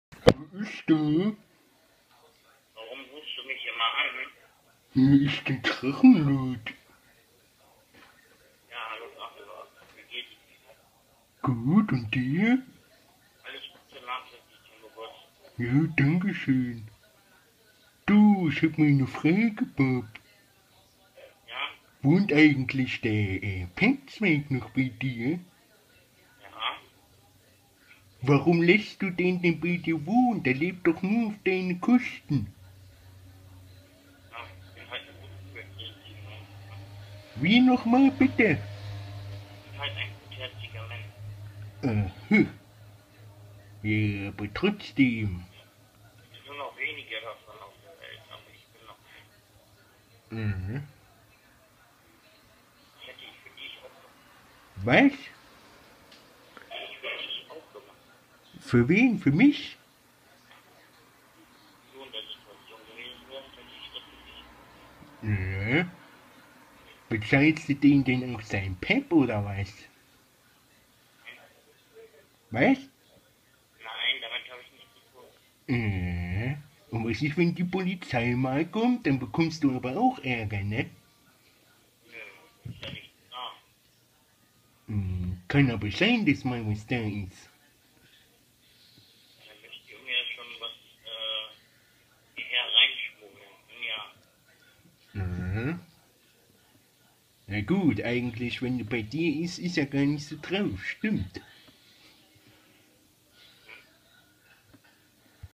Bob.TV ruft bei mir an 27.8.2017
War unvorbereitet, und wieso auch, deswegen ich am zappeln wohl, und dat